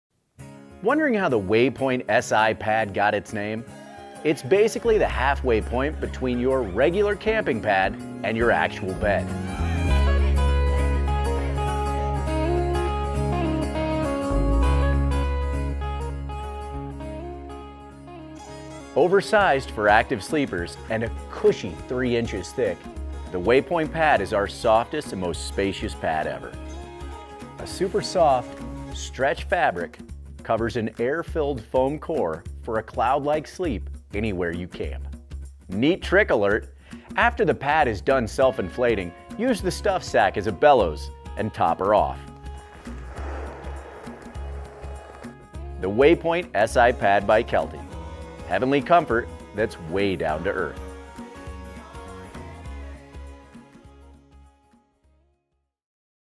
Kelty Waypoint SI Pad
Serious comfort for not-so-serious adventures.

Consider our new Waypoint the launching pad for your best car-camping dreams. Our biggest, thickest and softest pad, this spacious delight is basically the halfway point between a regular camping pad and your ACTUAL BED. With the Waypoint, comfort is king.